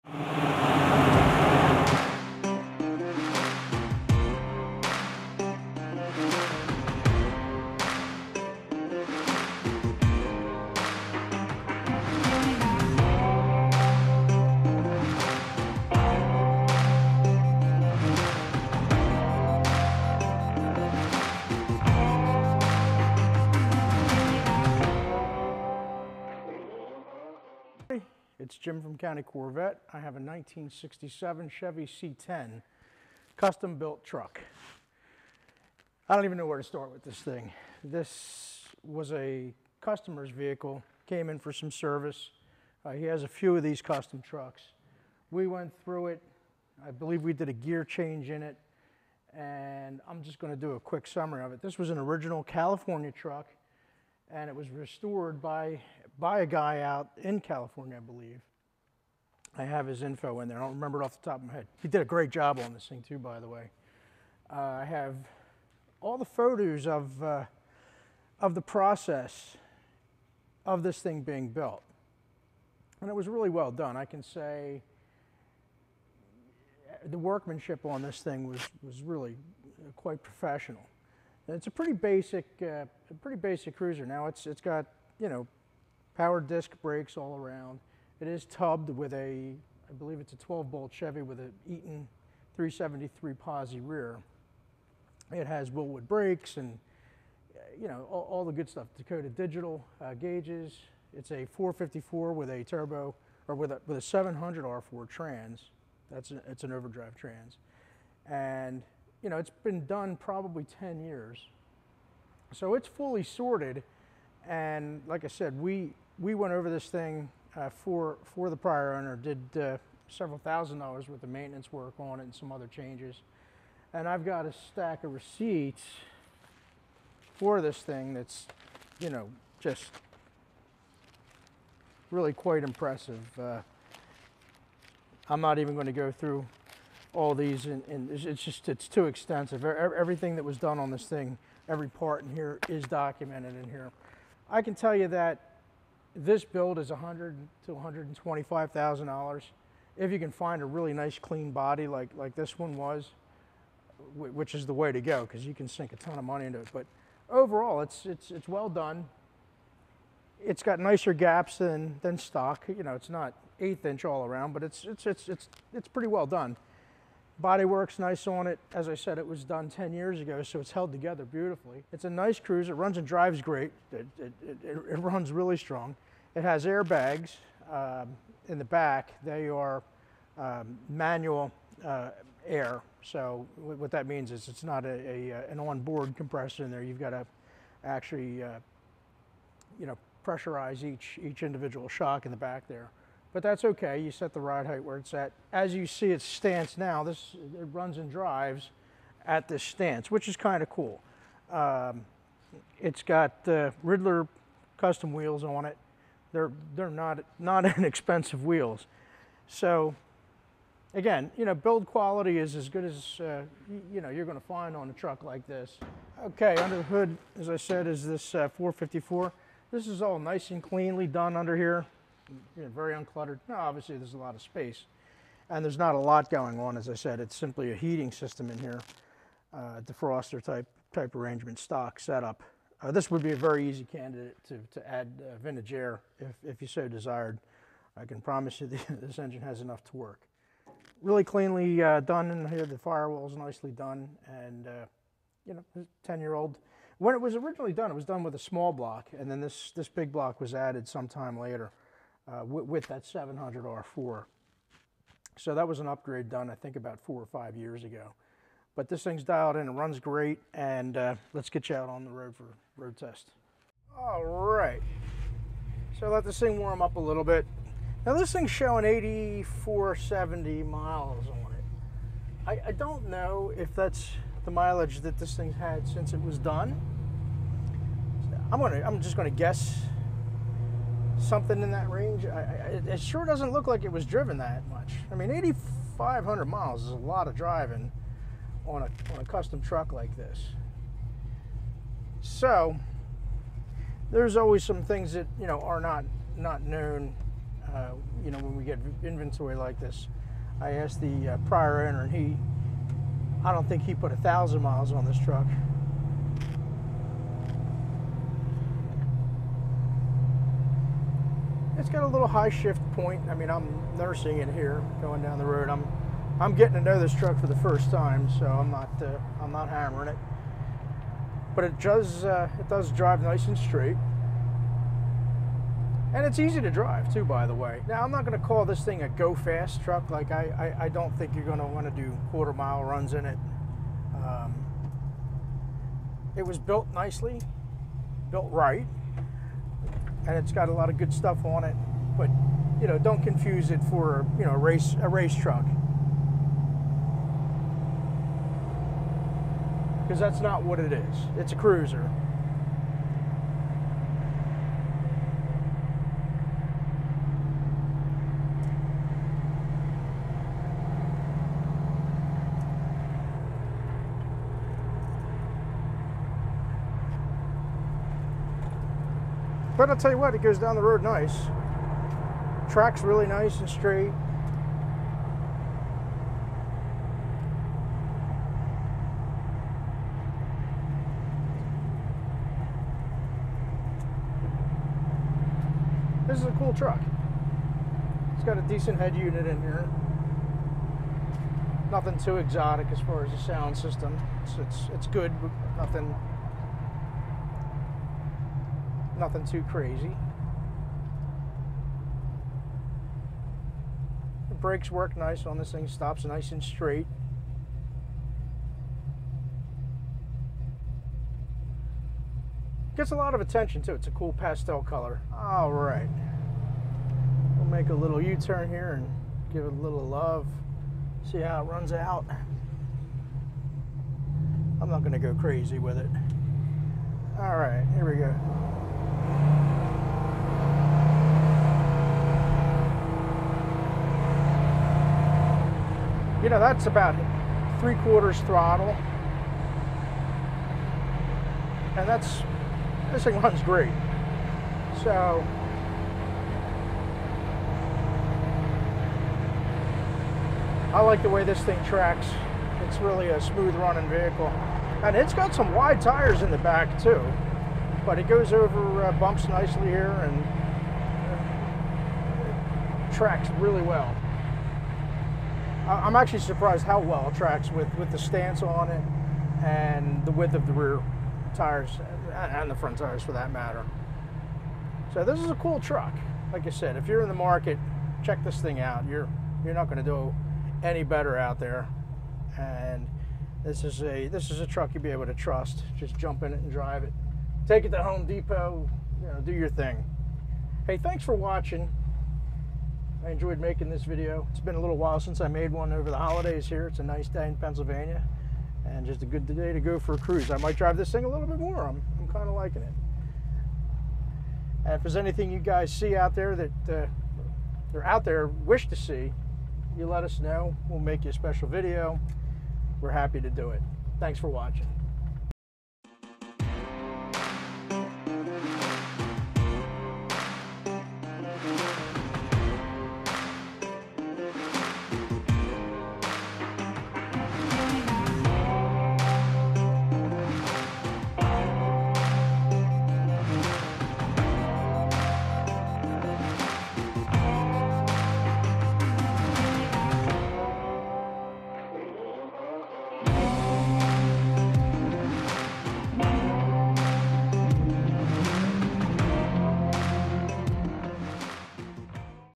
CHEVY C10 SHORT BED (CUSTOM TRUCK)
1967 Chevy C10 Short bed custom
Original clean California truck
Fully restored with modern upgrades
Powered by a custom built 454 big block Chevy engine
Probuilt 700 R4 Automatic transmission
Tubbed and narrowed custom rear with with 3.73 Eaton Posi unit
Power steering , power brakes, tilt wheel, Dakota digital gauges
Riddler custom wheels.
The body and paint workmanship on this truck is first class as it the custom chassis work
Estimated to have about 2500 miles on rebuild
This truck was very well built with a pile of receipts, extensive build documentation including photos
and serviced and sorted by County Corvette with no expense spared. You could not build this truck for less than $125 to $150K!
A fantastic looking and driving Chevy C10!

$59900.00